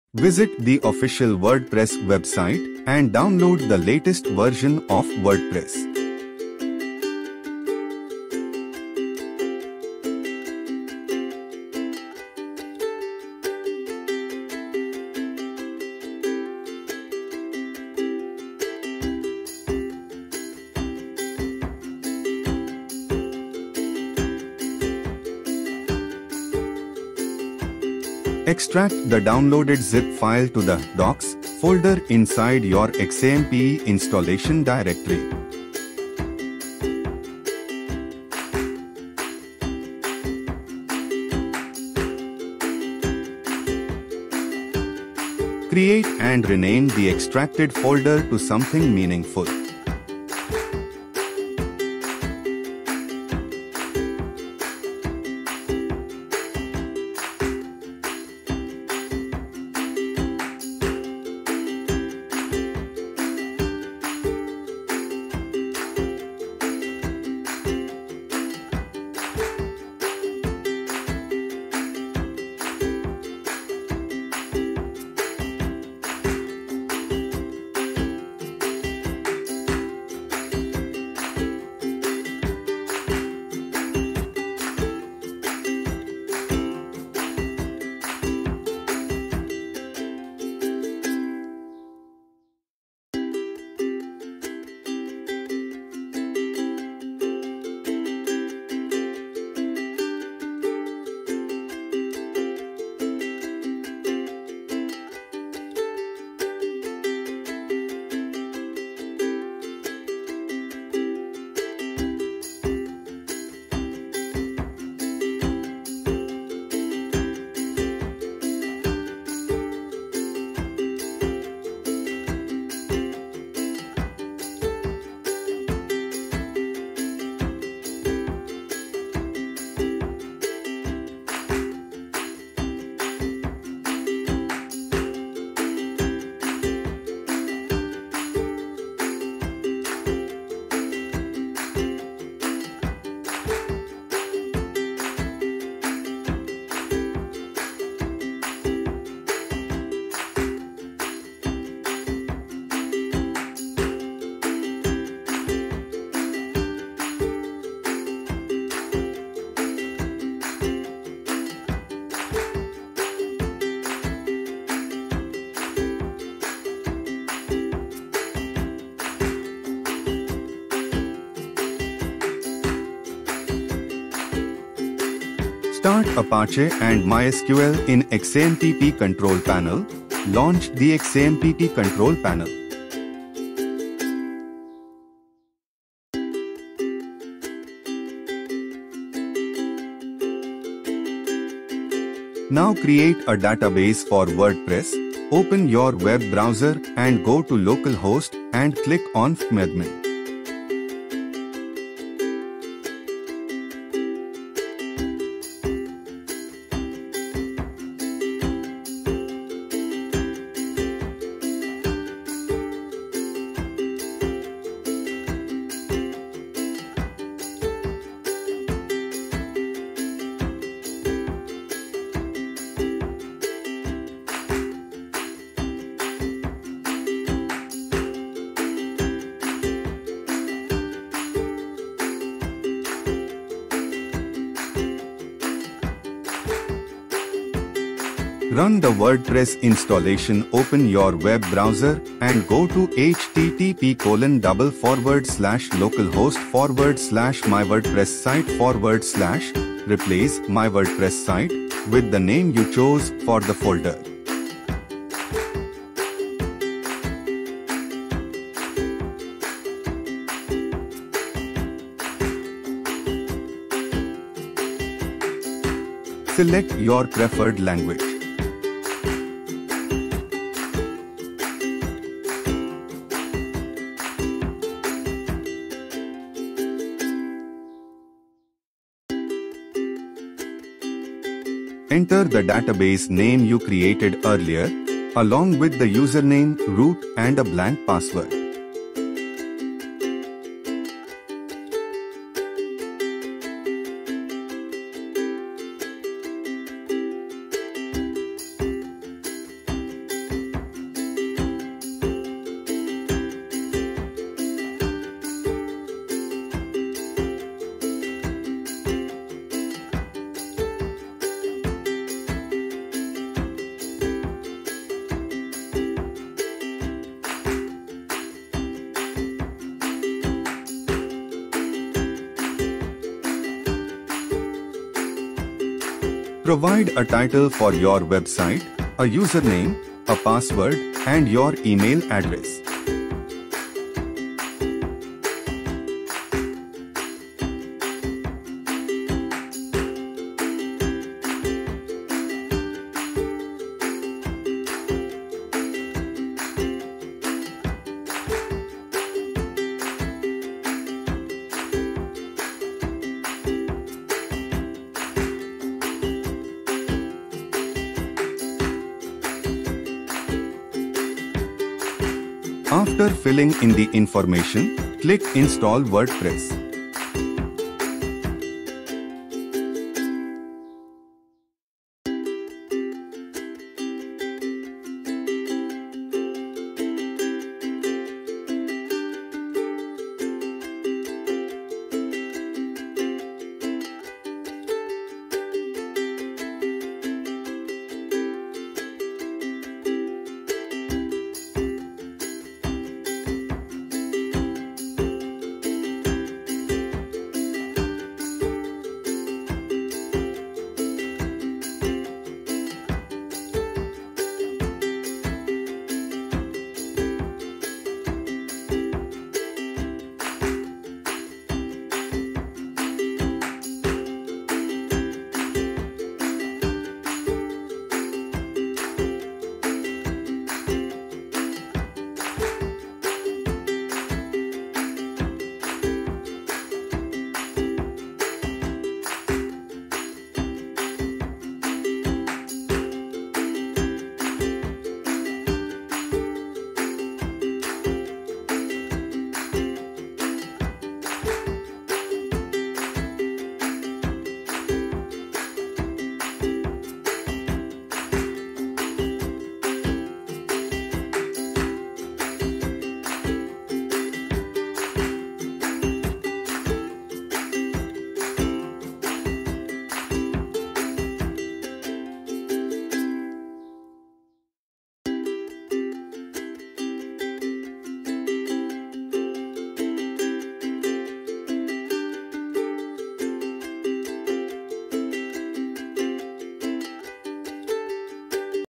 How to Install WordPress in Xampp Localhost on Windows 10/11 [ 2024 Update ]
Hey everyone! In this video, I'm going to give you a step-by-step tutorial on how to install WordPress on Windows 10/11 computer and use it for free. We'll be covering everything you need to know, including setting up XAMPP Localhost server on Windows 10/11 system. This is a complete guide to getting WordPress up and running on Xampp Localhost, so let's get started!

Topics Covered in this Video:
How To Download WordPress on Localhost
How to Start Apache and MySQL in XAMPP Control Panel
How to Create a Database for WordPress
How to Run the WordPress Installation